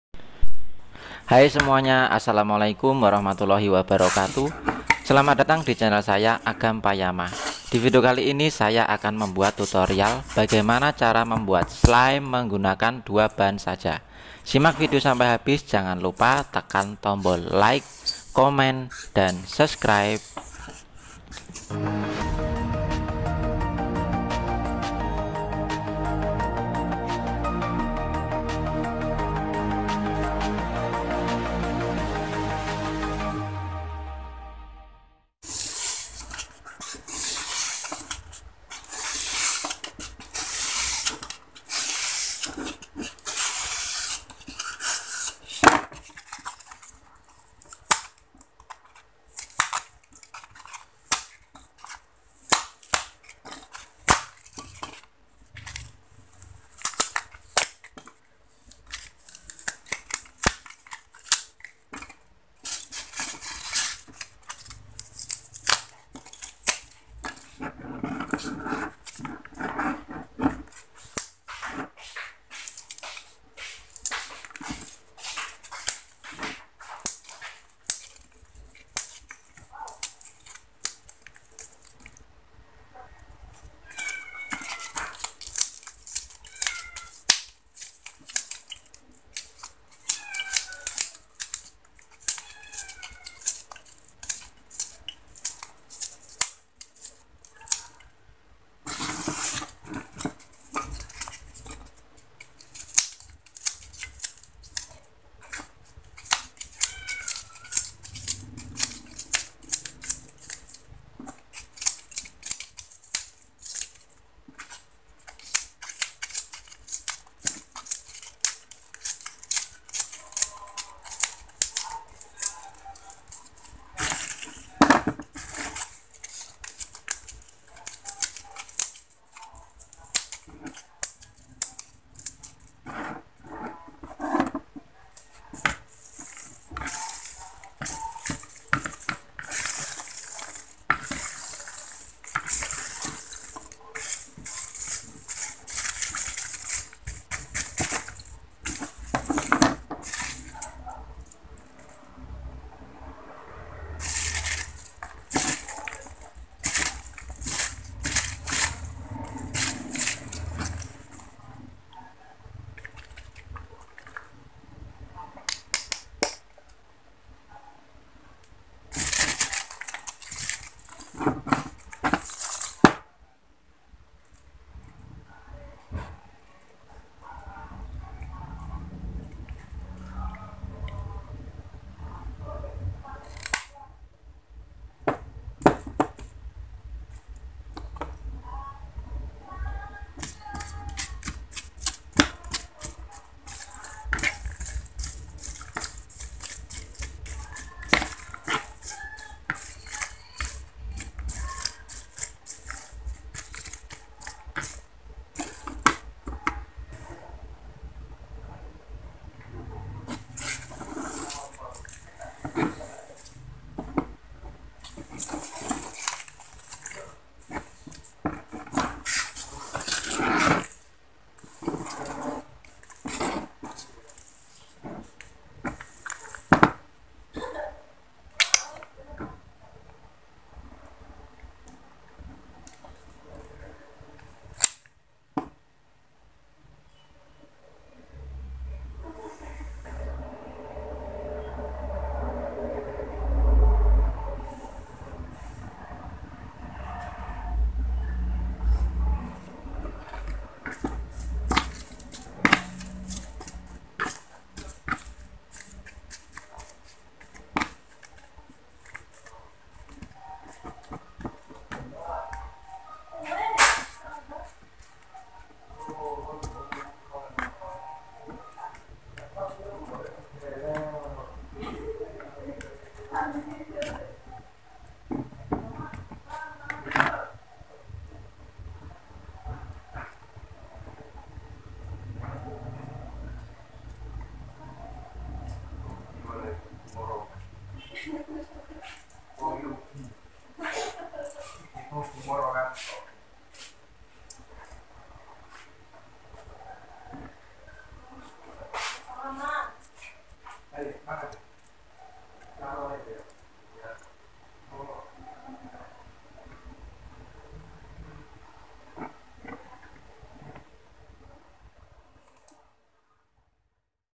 Cara membuat slime tanpa lem tanpa gom tanpa borak tanpa sabun || 2 bahan
Selamat datang di chanel saya agam payama, di vidio kali ini saya akan membuat tutorial bagaimana cara membuat slime tanpa lem tanpa gom tanpa borak tanpa sabun cair.

Simak vidio sampai habis ya.

Jadi disini aku akan membuat slime dengan bahan stearofoam aja.